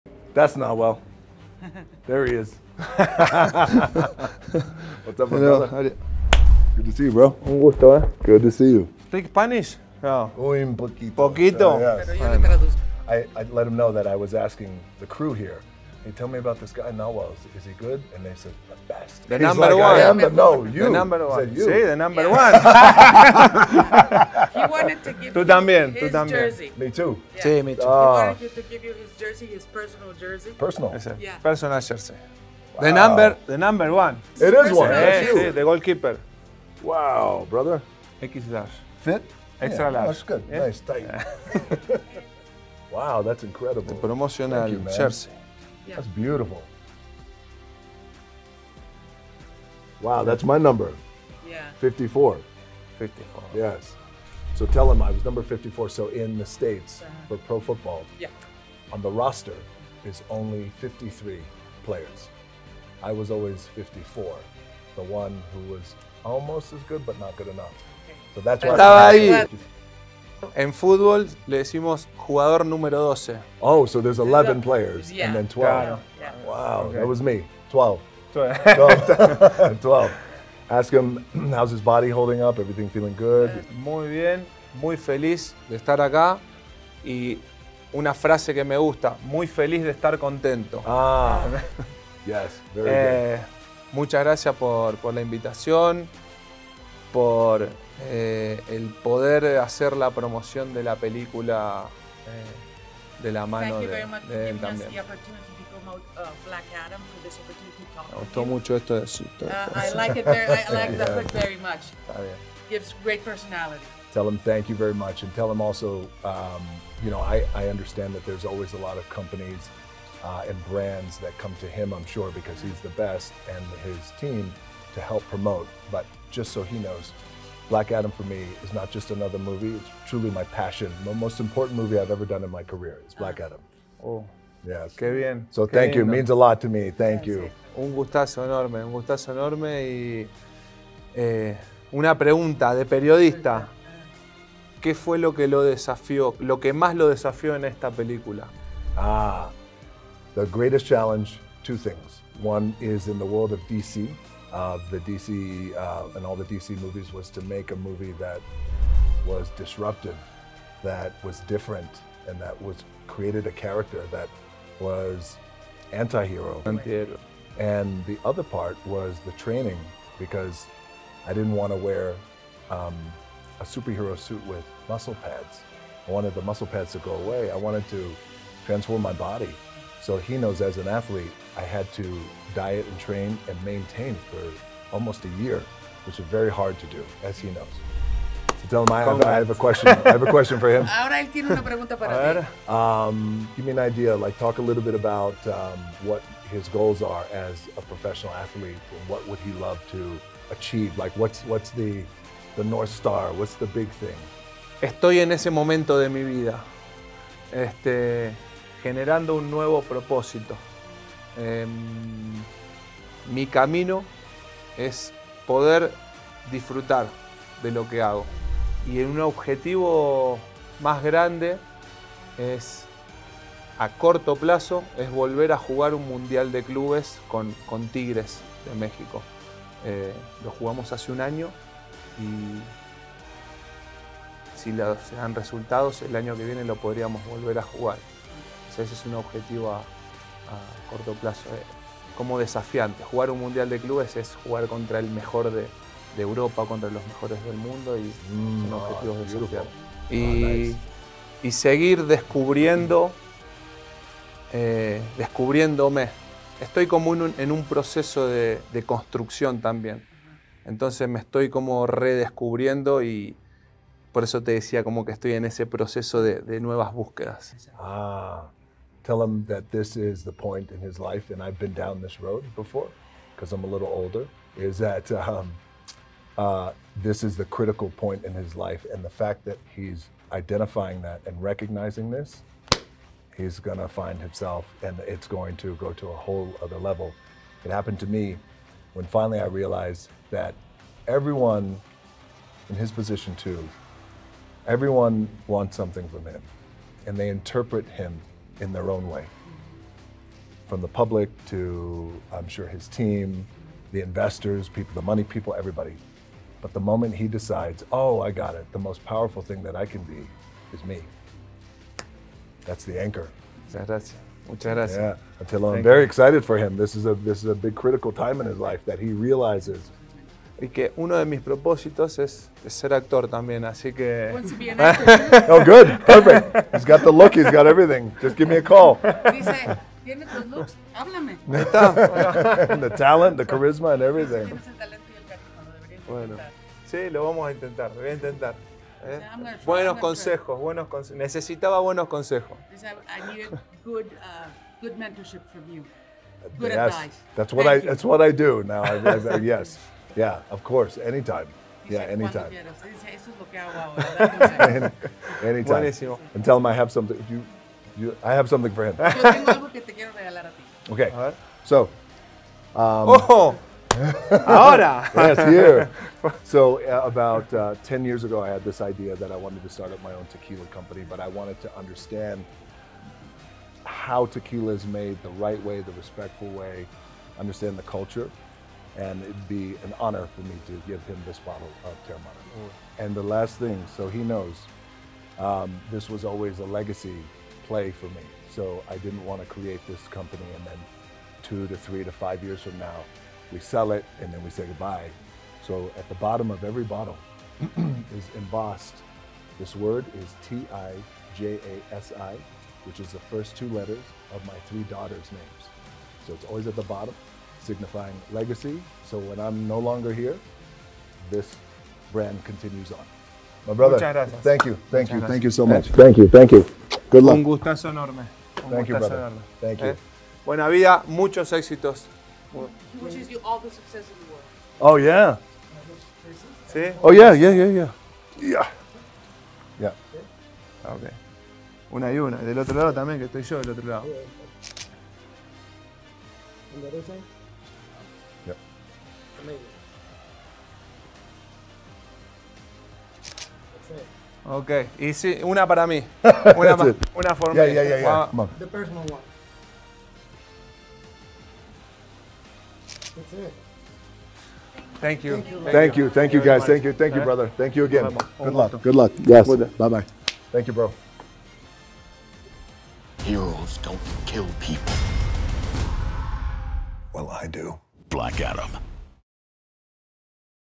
⚡️ Black Adam X Tigres 🐯 | Dwayne Johnson y Nahuel
¡Imperdible encuentro entre un crack de la cancha y un crack de la pantalla grande! ¡Disfruta esta charla entre Nahuel Guzmán y The Rock!

BlackAdam 20 de octubre, solo en cines.